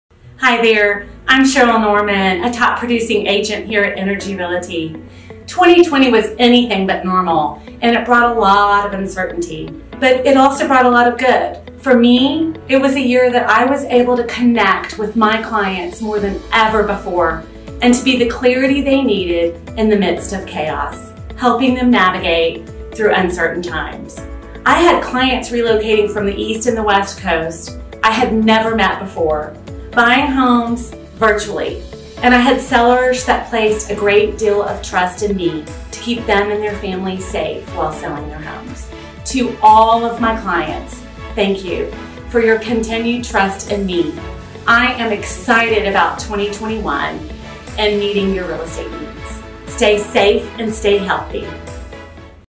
Agent Stats 2020 - Cheryl Norman
Energy Realty agents are thecreamofthecrop, and we are so proud of all of them. A big shout out to Cheryl Norman for an amazing year in 2020. She worked incredibly hard and always look after the best interest of her clients.